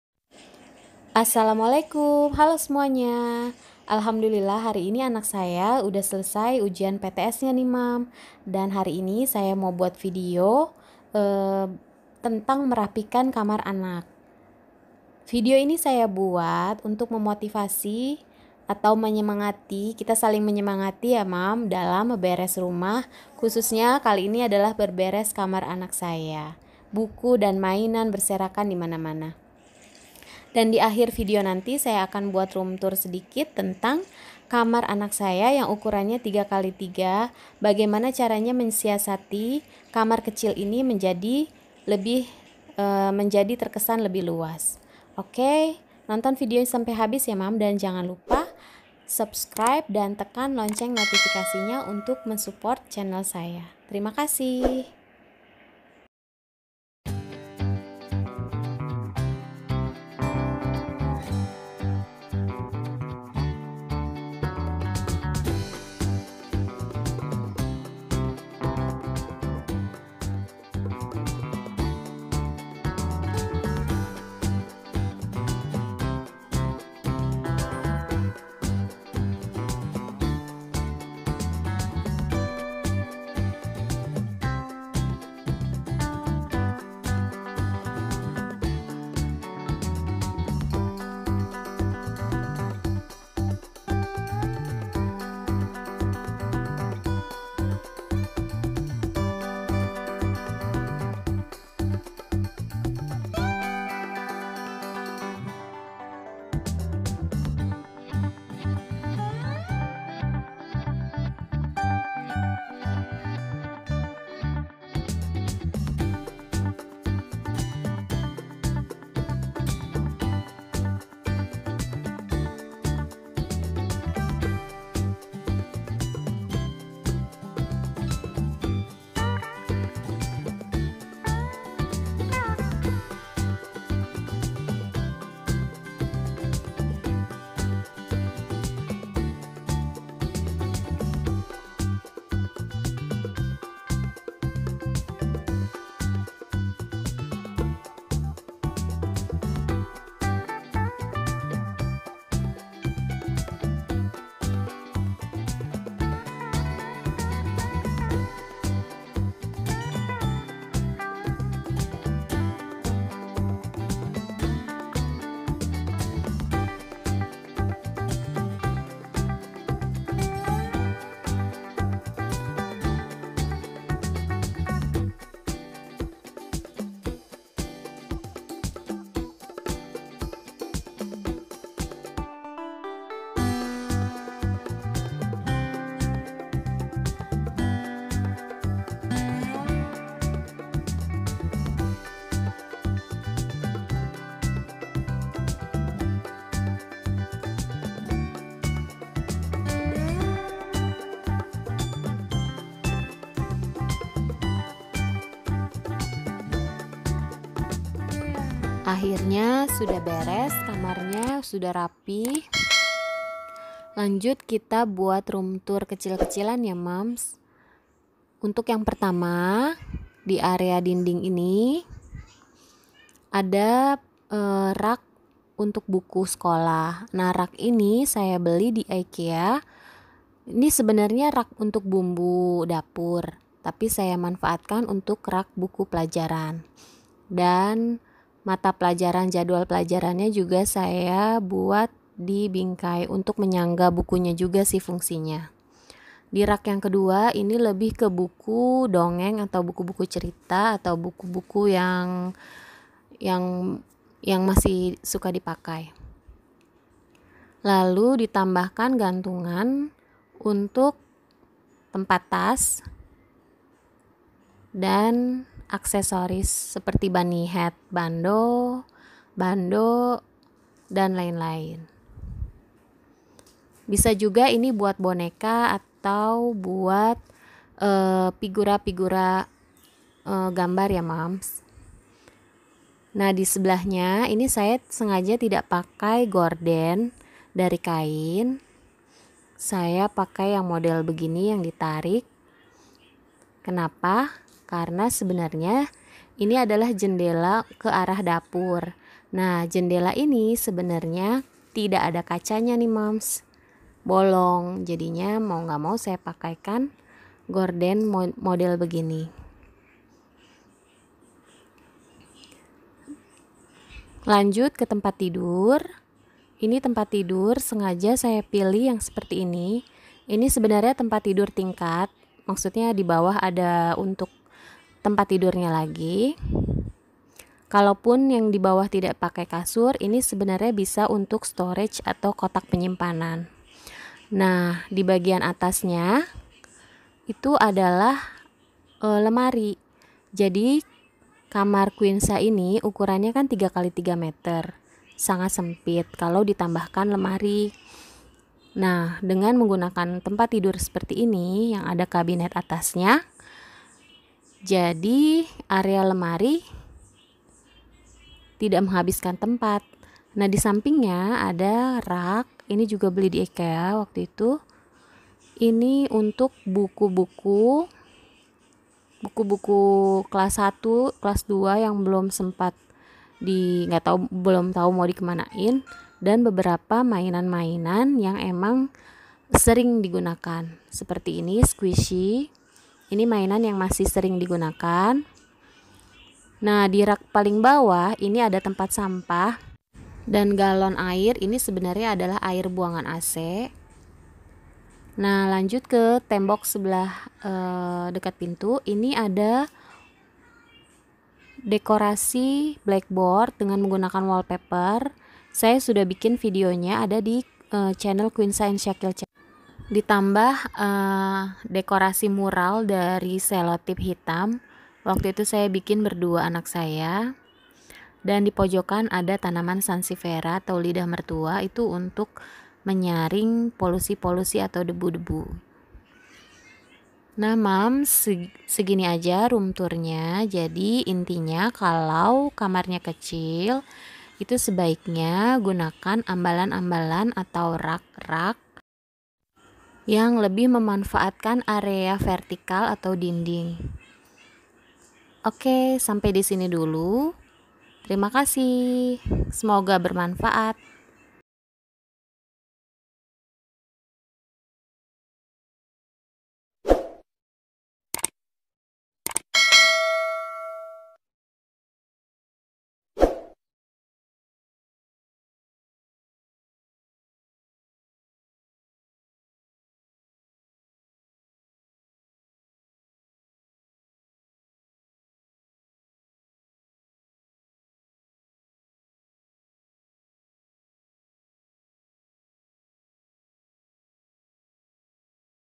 CLEANING ROOM - BEBERES KAMAR ANAK | ROOM TOUR KAMAR ANAK 2020
Assalamualaikum

Hai semua..
Kali ini saya membuat video Cleaning Room atau beberes kamar atau merapikan kamar anak saya. Tujuannya  untuk sharing dan memotivasi saling menyemangati dalam urusan beberes rumah.

Kamar anak saya berukuran 3 × 3 meter. Ukuran kamar anak yang tidak terlalu besar. Di video ini saya juga membuat room tour kamar kecil, kamar anak perempuan. Dengan penempatan beberapa rak dinding dan dekorasi yang sesuai membuat kamar terkesan luas, nyaman dan rapi.

Nonton video saya yang lain ya moms..

Video Dekorasi Ruang Keluarga Konsep Tropical | Room Tour Ruang Keluarga - Room Tour Indonesia 2019

Semoga video ini bisa menjadi inspirasi dan bermanfaat.

Tidak ada maksud pamer atau riya, cuma mau sharing dan berbagi ide. Mohon maaf kalau banyak kekurangan.

Terima Kasih

This time I made a video about Cleaning Room - Cleaning my daughter's room. I made room tour too. My daughter's room is about 3 × 3 meters in size. The size room is not too big. The placement of several wall shelves and decorations make the room seem spacious, comfortable and neat.